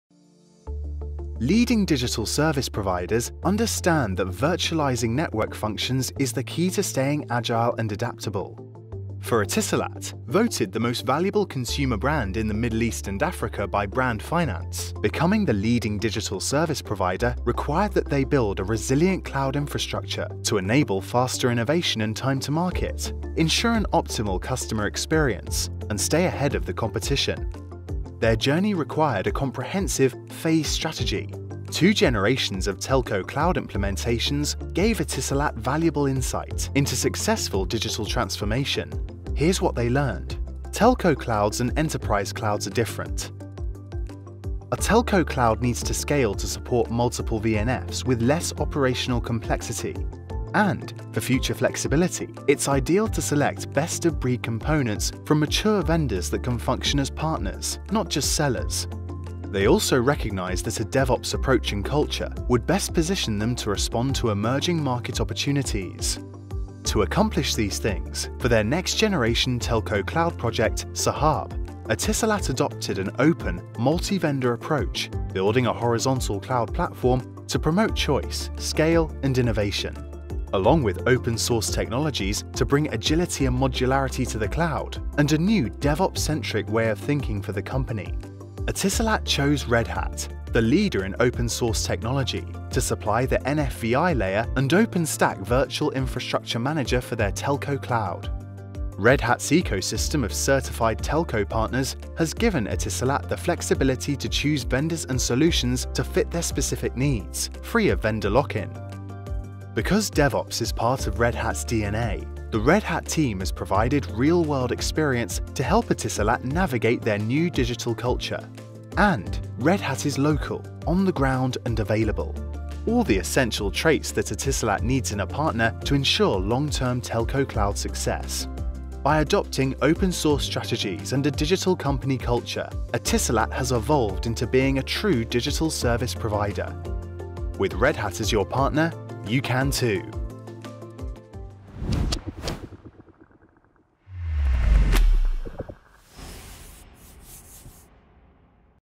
Etisalat SAHAB - sound advice for building a modern telco cloud
Etisalat, voted Most Valuable Consumer Brand in the Middle East and Africa by Brand Finance, has progressed through multiple generations of telco cloud. Learn about their key considerations for staying agile while reducing cost and network complexity.